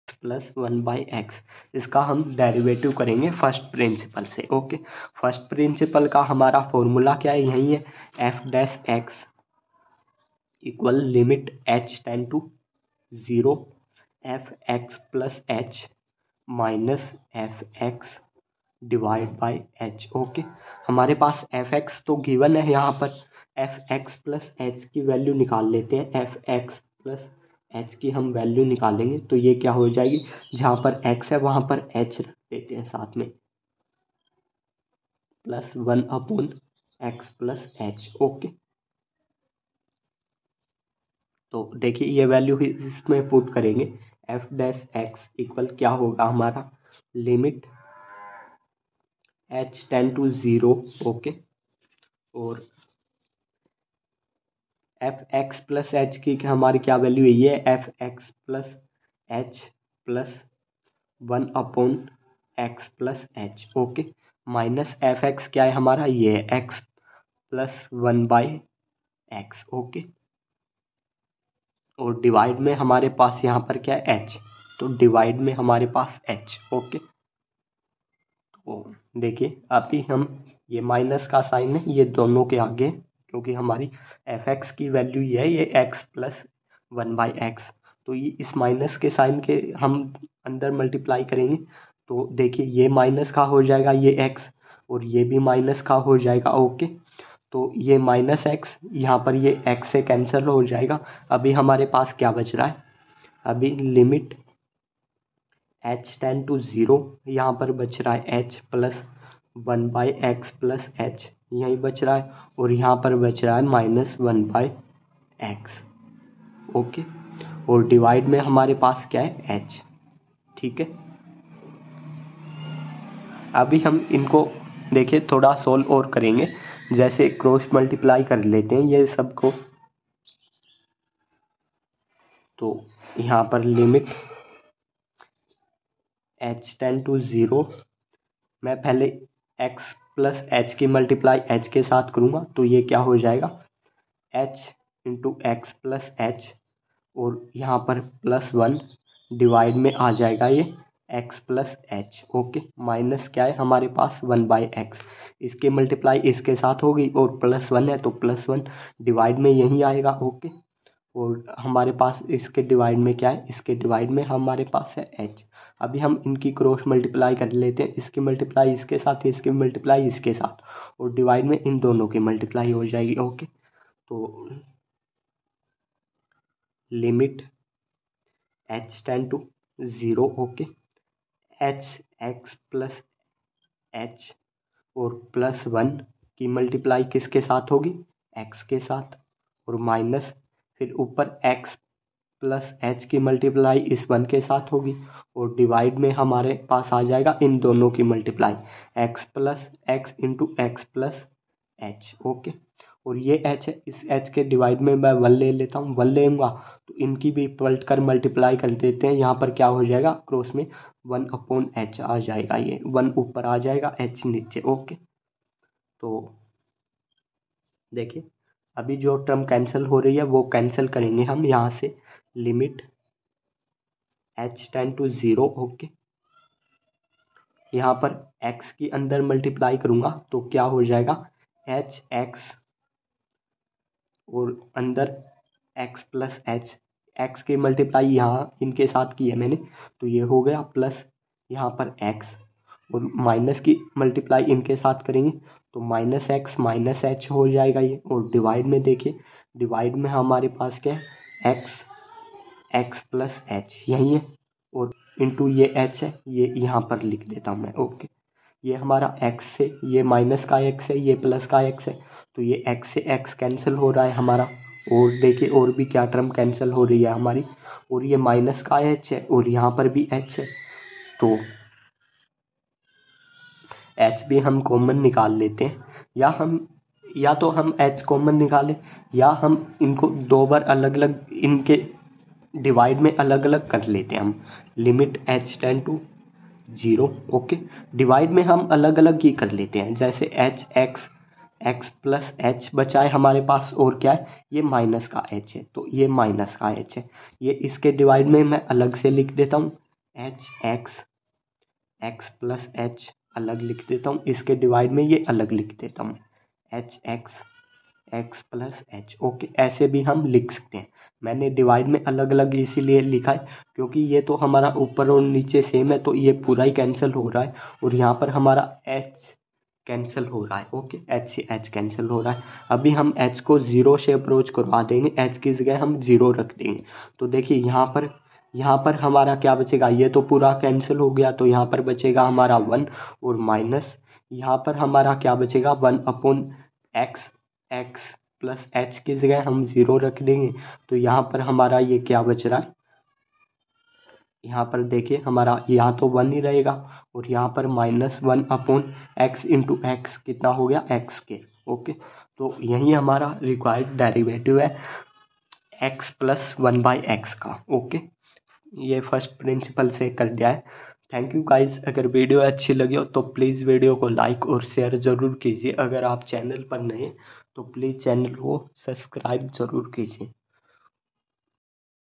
class 11 chapter 12 Example 19 ii derivative of x+1/x using first principle
namaskar

class 11 chapter 12 example 19 (ii)
 derivative of x+1/x using first principle